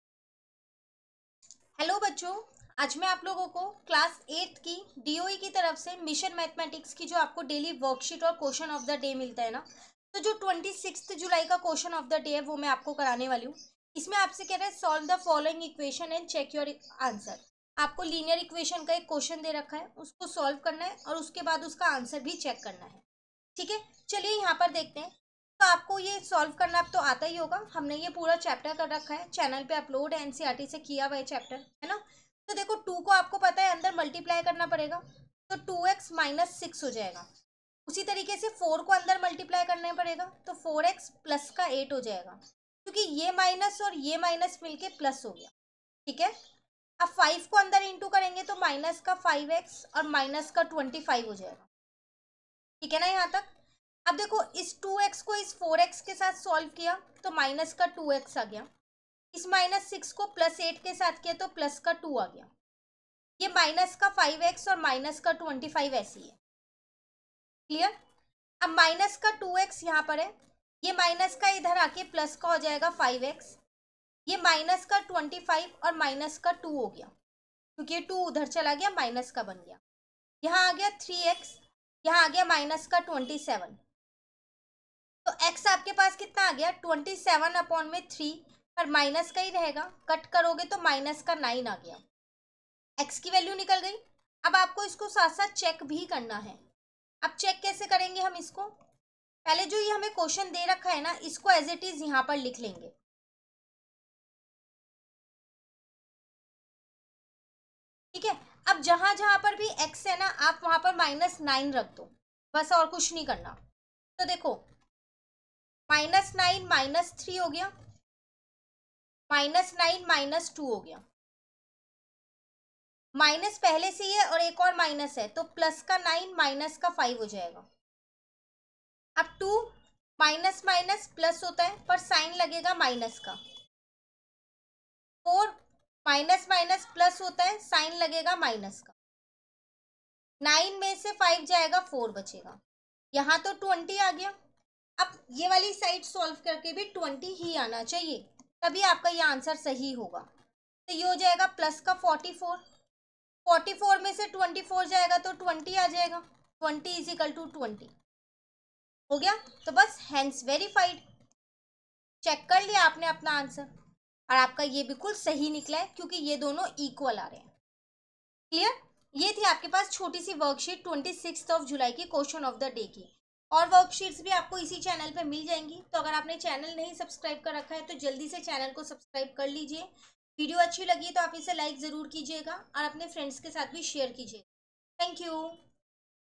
Class 8 Maths | DOE Mission Mathematics Worksheet (Question of the day) | 26 July 2023
In this video, I have solved DOE Mission Mathematics Worksheet (Question of the day) of Class 8 Maths.

Textbook credits/Source: DOE

Chapterwise Solutions of 8th Class Maths NCERT Book